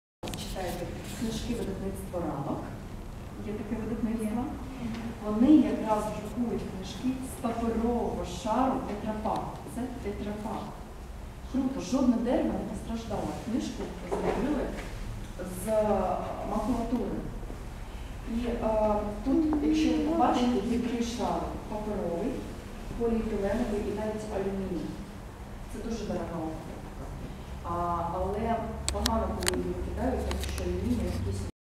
"Україна без сміття"
Україна без сміття - екскурсія на станцію роздільного сортування сміття в м. Києві. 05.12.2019
Перша громадська сортувальна станція «NO WASTE recycling station» разом з партнером проекту ТМ “Яготинське ”Молочний альянс" створили можливість для шкіл Києва ти Київщини потрапити на безкоштовну екскурсію, яка зазвичай коштує 2000 грн.